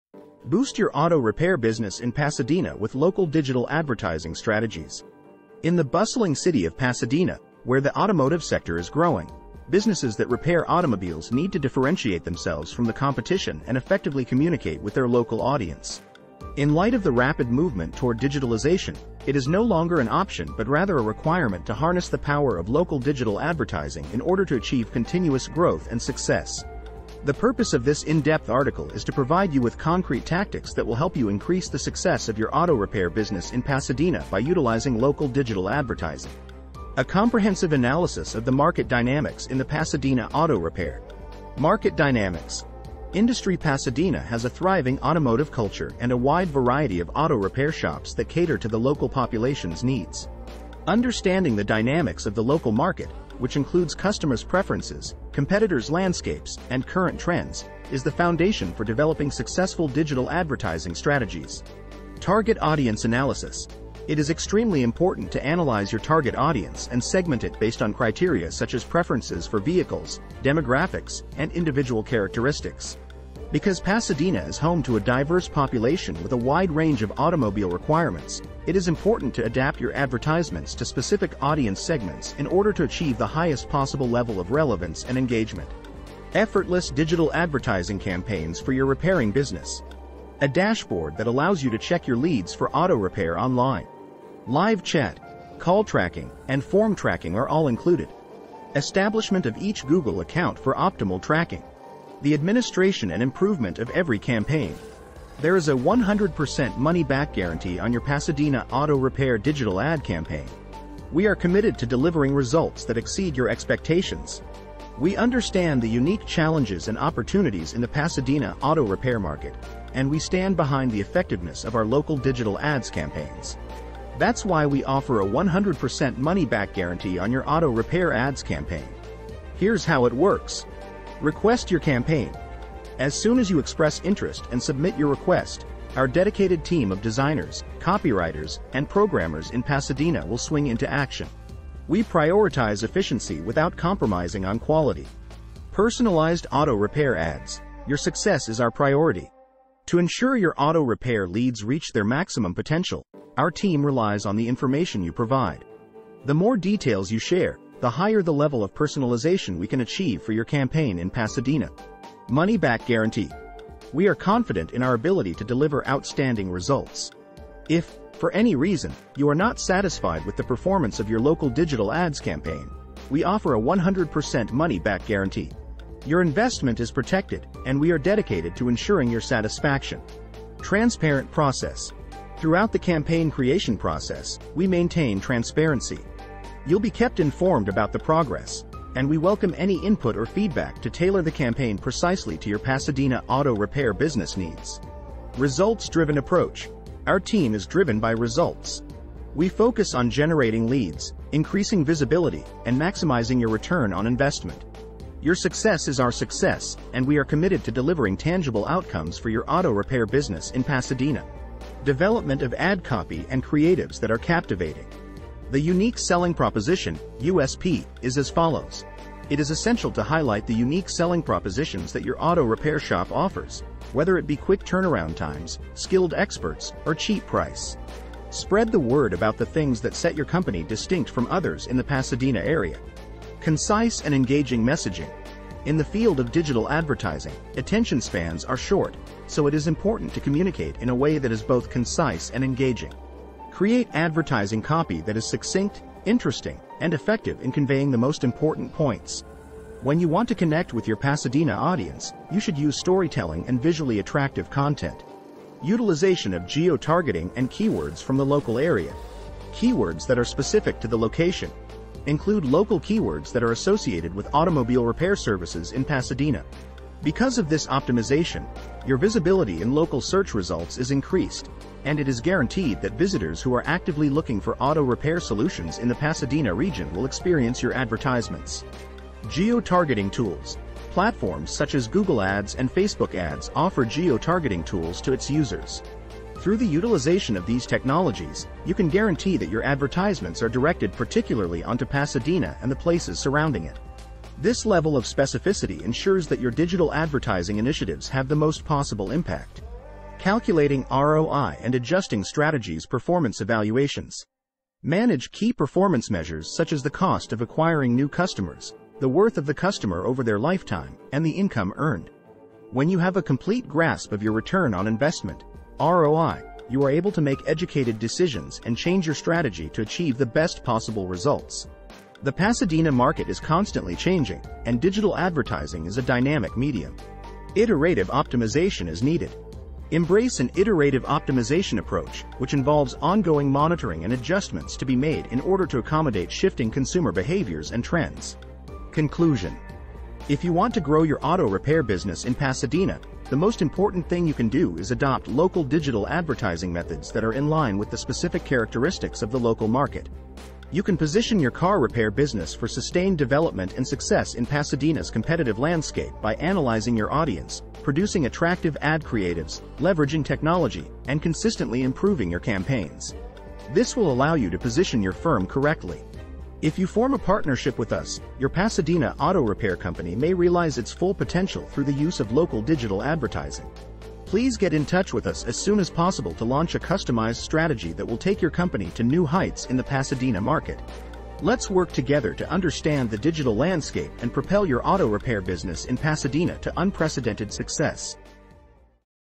Boost Your Auto Repair Business In Pasadena With Local Digital Advertising Strategies
Attention Pasadena auto repair shop owners! Are you ready to take your business to the next level? In this video, we're unleashing the power of local digital advertising and providing you with the ultimate guide to elevate your auto repair business in Pasadena.

With the ever-growing competition in the automotive industry, standing out is crucial. That's where local digital advertising comes into play. Whether you're a newcomer or an established name, targeted digital advertising can be the game-changer you need to attract more customers and drive growth.

Join us as we delve into proven strategies for leveraging local digital advertising to boost your auto repair business in Pasadena. From targeting the right audience and choosing the most effective platforms to crafting compelling ad creatives and optimizing your campaigns for maximum ROI, we'll cover everything you need to know to succeed in the digital landscape.

Don't let your competitors race ahead - watch now and discover how local digital advertising can fuel the success of your auto repair business in Pasadena!